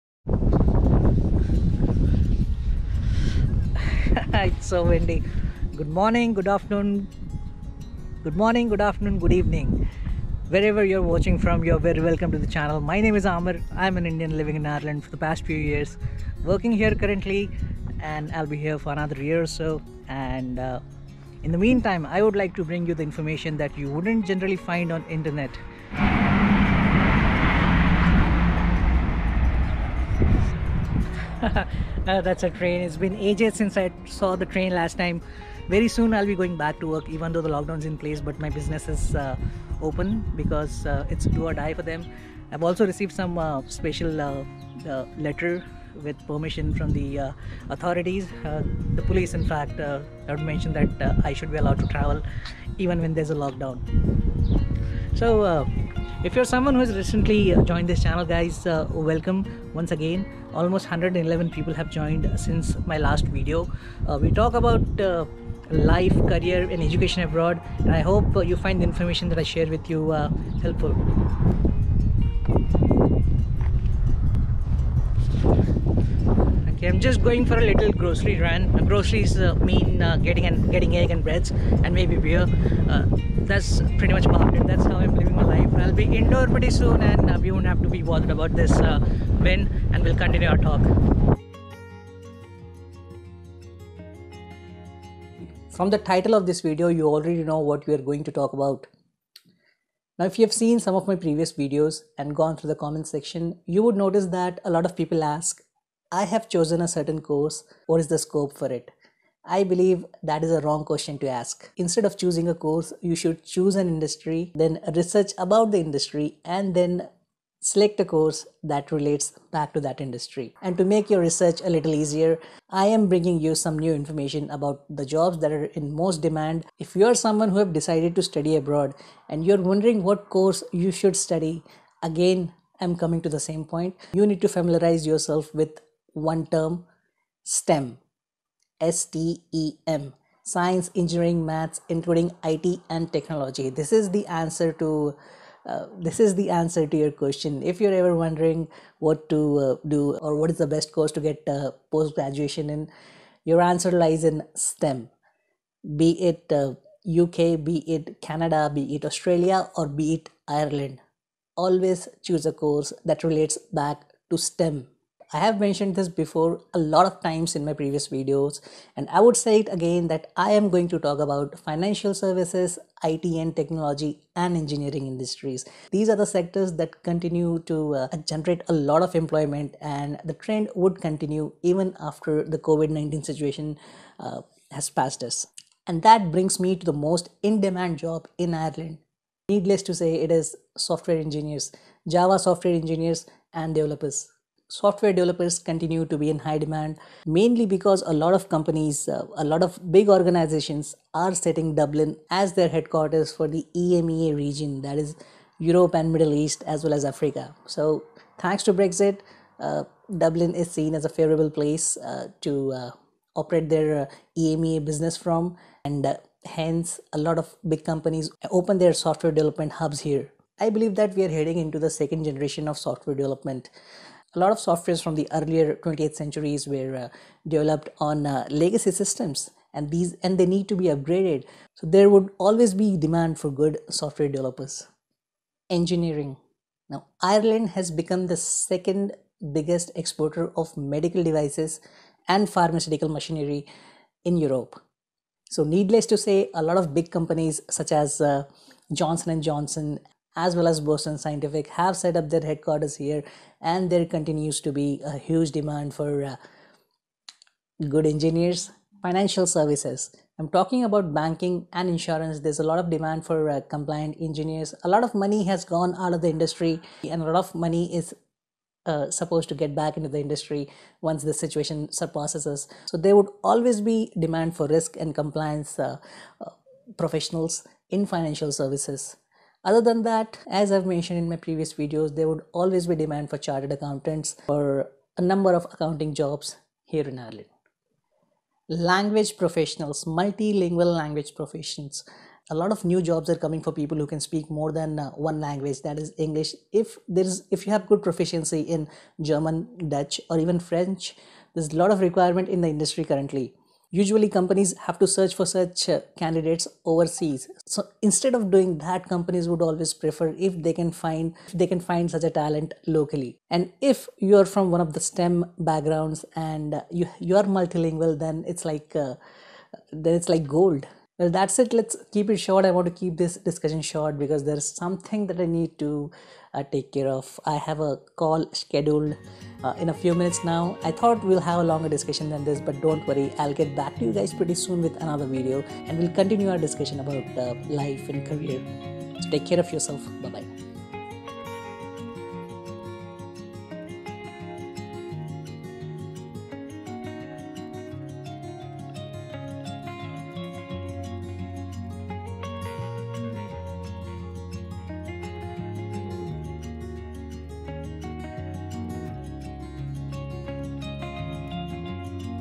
Most in demand jobs in Ireland
What careers are in high demand in Ireland? In the current candidate short market there is an increased demand for skilled talent across a number of industries including Financial Services, Technology and Engineering.
Yet again, the most persistent skills gaps continue to be STEM-related careers – Science, Engineering and Maths, including IT and Technology.

In particular, people who have the expertise and skills for the following jobs are highly in demand in Ireland right now:

Technology: Java Software Engineers / Developers
Software developers and engineer continue to be in high demand as more companies choose Dublin as their EMEA HQ and open up software development centres here.

Java software engineers with React experience and who are data centre are particularly in demand. Also, those who have had exposure to monolith legacy apps/platforms are of high value to companies and will command attention in 2019.

Engineering: Automation Engineer
An automation engineer uses technology to improve and streamline a variety of manufacturing processes. As the pharmaceutical and medical device spaces grow rapidly in Ireland, so does the demand for automation engineering.

Ireland has become the second largest exporter of medical devices in Europe and they account for 10% of all Irish exports. Companies including Boston Scientific and Johnson & Johnson are big in this space and creating demand for engineering professionals countrywide.

Financial Services – Risk & compliance professionals within Asset Management
A number of the world’s leading investment banks and asset management companies are opening new functions in Dublin, particularly within the Risk and Compliance space.

Barclays and Bank of America Merrill Lynch are just two prominent examples of this, illustrating the rising demand.

Insurance – compliance professionals
On a whole, Ireland’s appeal as a centre for insurance excellence has grown, attributed in part to the EU’s Insurance Framework Directives which allows insurance companies in Ireland to carry out business in all EU member states.

Candidates specializing in compliance are one area in greatest demand and will be able to seek premium salaries in 2019.

This area of Insurance has been known to have a shortage of experienced and knowledgeable candidates, often pushing companies to look overseas for suitable talent.

Languages – Multilingual professionals
As Ireland’s interests in the international market become increasingly apparent, the demand for multilingual talent to support global companies such as Google, Salesforce and Twitter, is growing.

Languages proving most desirable include German, Dutch and French, with companies often having to search overseas to find suitably qualified candidates. Candidates in Ireland who are proficient in any of these languages can expect good job opportunities in coming years.